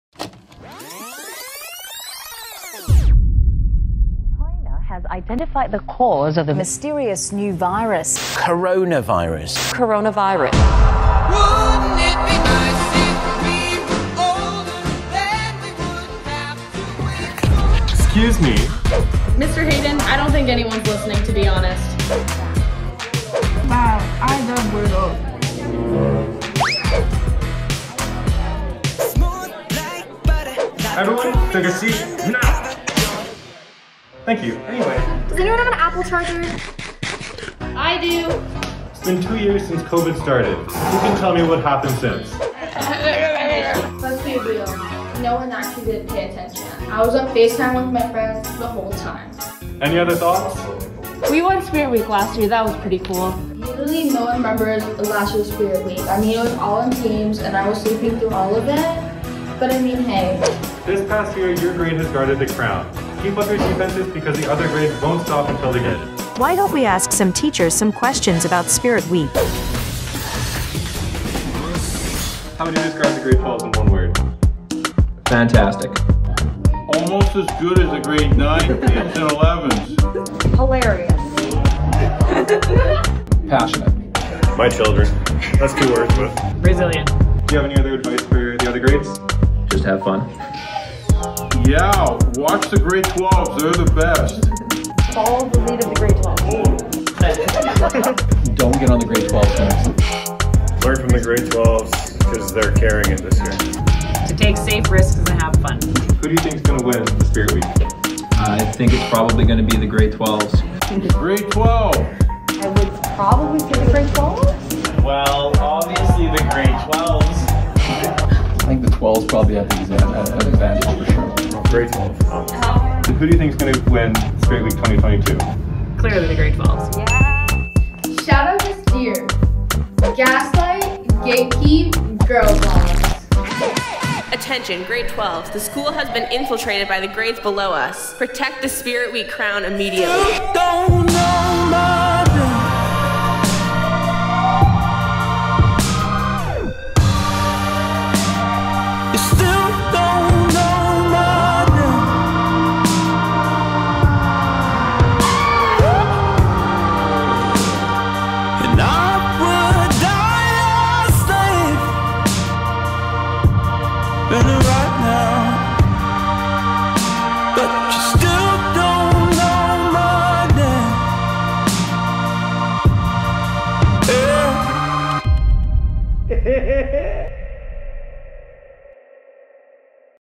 Grade 12 Video - GCI Spirit Week 2022
Glenlawn Collegiate Grade 12 Spirit Week Grade Video
Grade Video won 1st Place

I forgot to credit the Announcement Voice: Sadie
Juliana

Thank you to everyone who participated in this video 💖